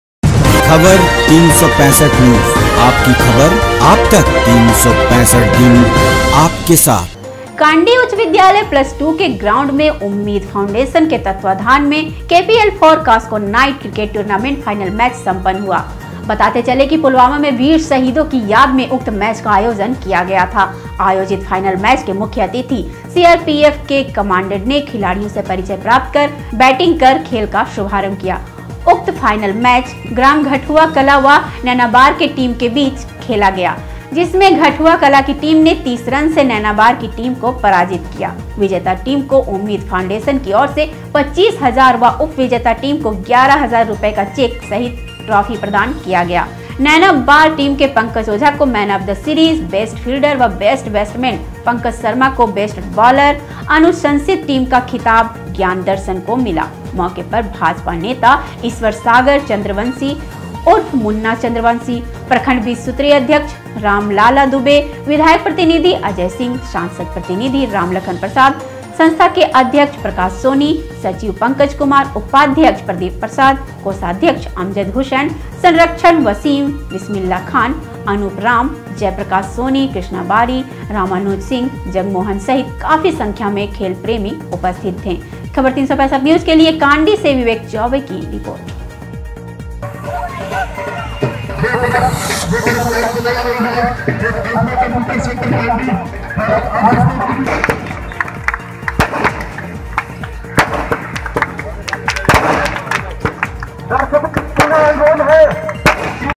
Kandi : घटहुआँ कला की टीम" ने "नैनाबार की टीम" को 30 रन से किया पराजित
कांडी(गढ़वा) : उच्च विद्यालय प्लस 2 के ग्राउंड में उम्मीद फाउंडेशन के तत्वावधान में केपीएल फोर कास्को नाईट क्रिकेट टूर्नामेंट फाइनल मैच सम्पन्न हुआ।बताते चलें कि पुलवामा में वीर शहिदों के याद में उक्त मैच का आयोजन किया गया था।आयोजित फाइनल मैच के मुख्य अतिथि सीआरपीएफ के कमांडेंट ने खिलाड़ियों से परिचय प्राप्त कर बैटिंग कर खेल का शुभारंभ किया। उक्त फाइनल मैच ग्राम-घटहुआं कला व नैनाबार के टीम के बीच खेला गया, जिसमें घटहुआं कला की टीम ने 30 रन से नैनाबार की टीम को पराजित किया । विजेता टीम को उम्मीद फाउंडेशन की ओर से 25 हजार व उप विजेता टीम को 11 हजार रूपये का चेक सहित ट्राफी प्रदान किया गया।नैनाबार टीम के पंकज ओझा को मैन आफ द सीरीज , बेस्ट फील्डर व बेस्ट बैस्टमैन , पंकज शर्मा को बेस्ट बाँलर , अनुशंसित टीम का खिताब ज्ञान दर्शन को मिला। मौके पर- भाजपा नेता-ईश्वर सागर चंद्रवंशी उर्फ मुन्ना चंद्रवंशी, प्रखण्ड बीस सूत्री अध्यक्ष -रामलाला दूबे , विधायक प्रतिनिधि- अजय सिंह , सांसद प्रतिनिधि-रामलखन प्रसाद संस्था के अध्यक्ष-प्रकाश सोनी, सचिव- पंकज कुमार , उपाध्यक्ष-प्रदीप प्रसाद, कोषाध्यक्ष-अमजद हुसैन, संरक्षक-वसीम, विस्मिला खान, अनुप राम, जय प्रकाश सोनी, कृष्णा बारी, रामानुज सिंह, जगमोहन सहित काफी संख्या में खेल प्रेमी उपस्थित थे। संवाददाता-विवेक चौबे kandi gadwa.